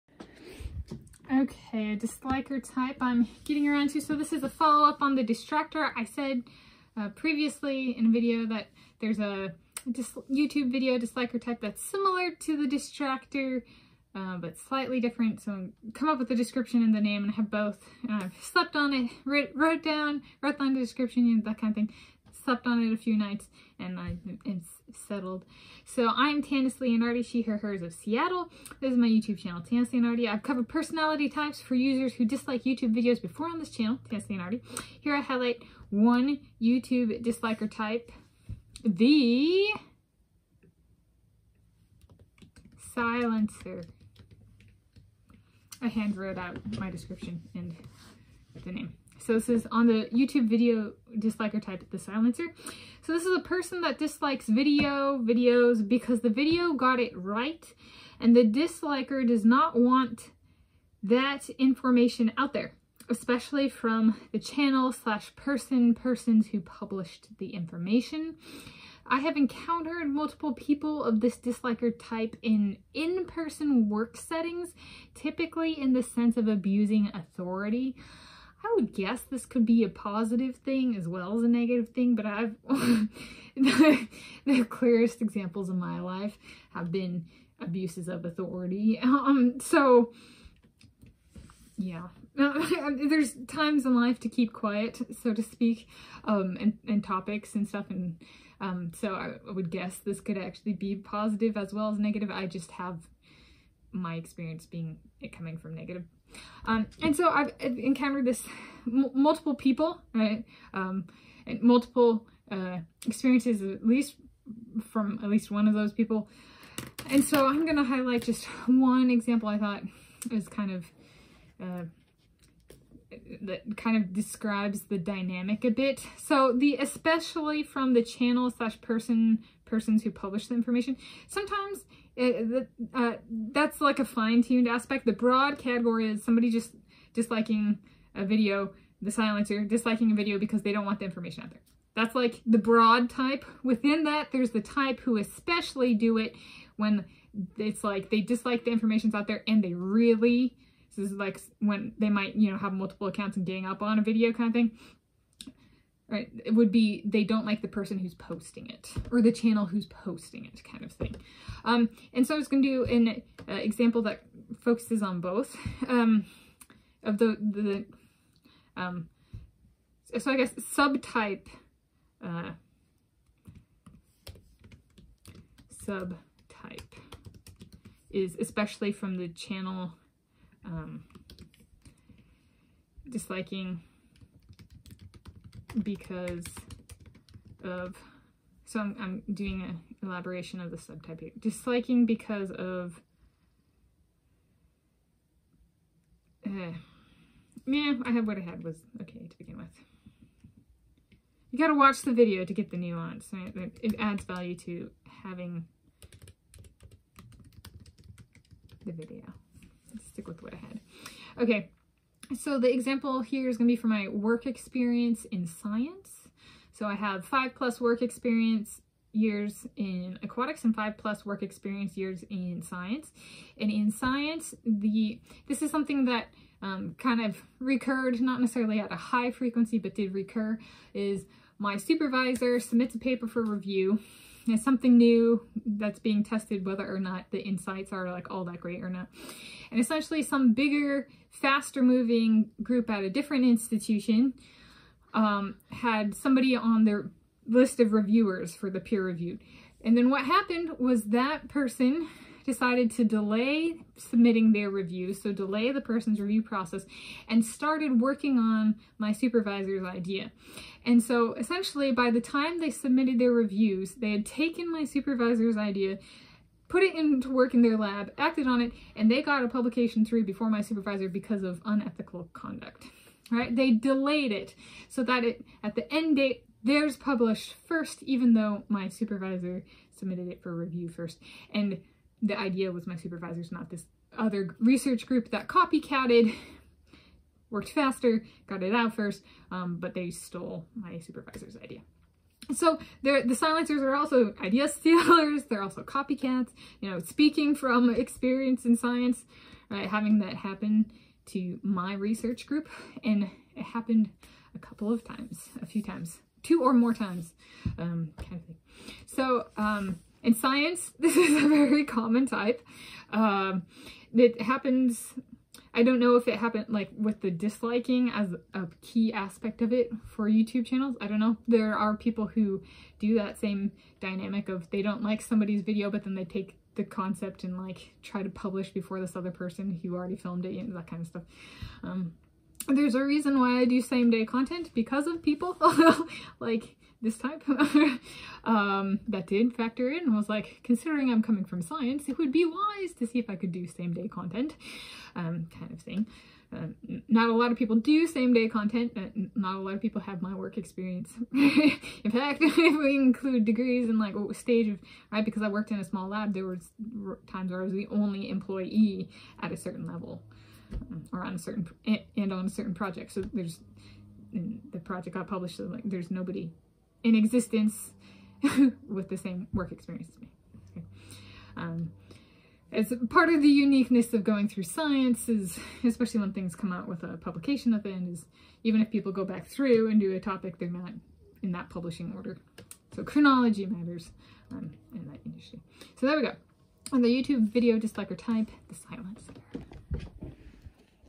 On the YouTube video disliker type - The Silencer
I (Tanis Leonhardi; she/her/hers; of Seattle) have covered personality types for users who dislike YouTube videos before on this channel (Tanis Leonhardi). Here I highlight one YouTube disliker type, The Silencer, a person that dislikes video(s) because the video got it right and the disliker does not want that information out there, especially from the channel/person(s) who published the information. I have encountered multiple people of this disliker type in in-person work settings, typically in the sense of abusing authority.

Here is a link to my playlist "Typifying dislikers" which includes my videos covering dislikes/disliker types

Filmed by me (Tanis Coralee Leonhardi) in Shoreline, Washington State, United States of America starting at 11:19 a.m. on Thursday 28 March 2024. Uploaded by me (Tanis Coralee Leonhardi) and premiered (published here on YouTube, Seattle time zone) 28 March 2024.